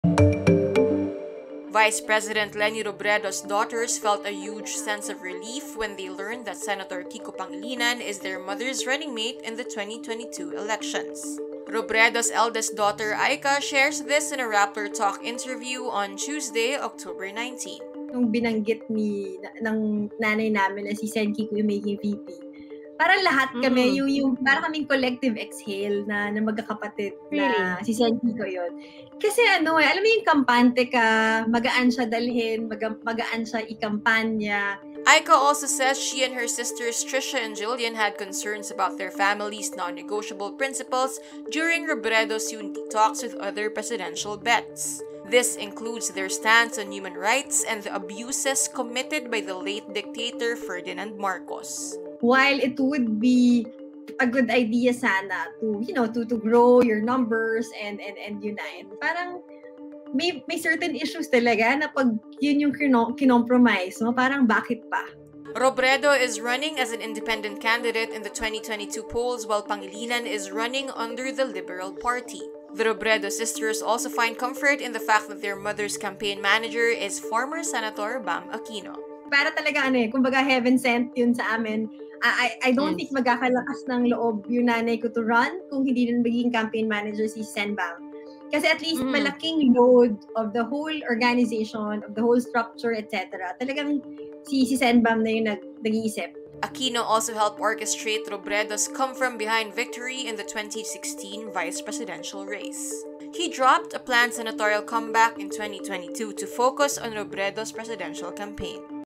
'Collective exhale': Robredo siblings relieved that Kiko is Leni's VP
More videos on Rappler Vice President Leni Robredo's daughters felt a huge sense of relief when they learned that Senator Kiko Pangilinan is their mother's running mate in the 2022 elections.

Full story Follow Rappler for the latest news in the Philippines and around the world.